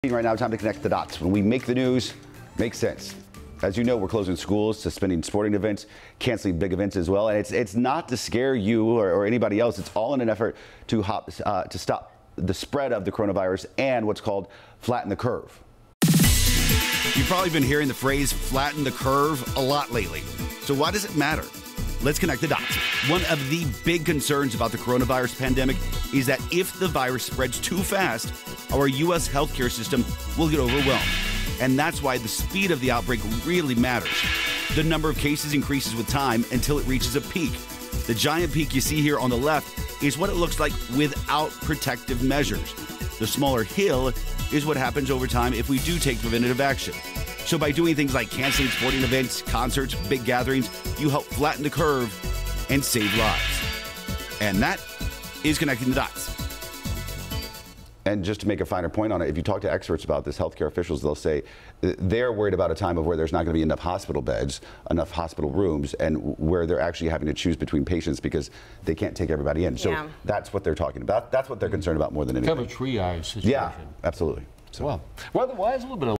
What it means to 'flatten the curve' on coronavirus
By now, you've probably heard the phrase "flattening the curve." Here's what that means and why it's so important to fight the coronavirus. It's the reason experts say we should continue to avoid major events, cancel sports and even suspend the NBA season.